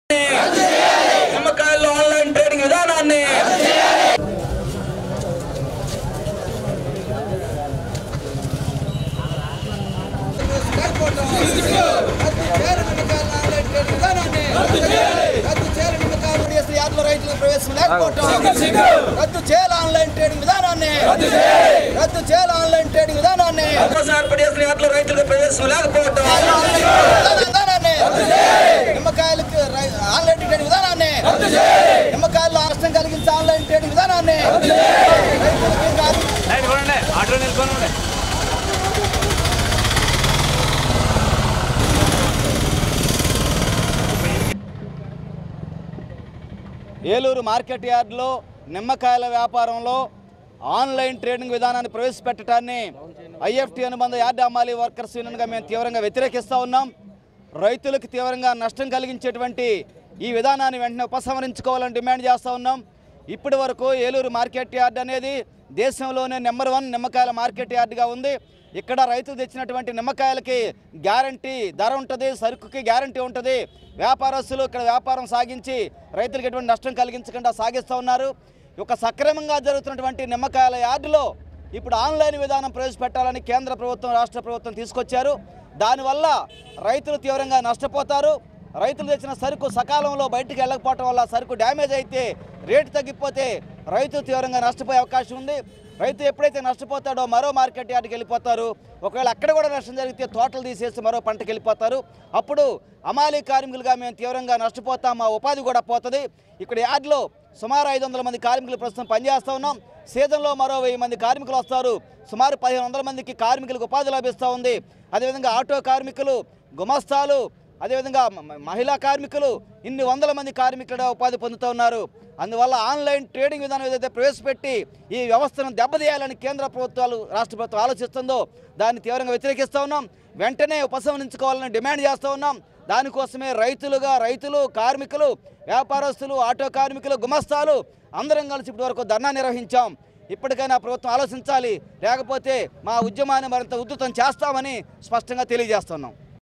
మీకు నిమ్మకాయలు కావాలా...మారి మాకేంటి
lemon marchant workers dharna at west godavari district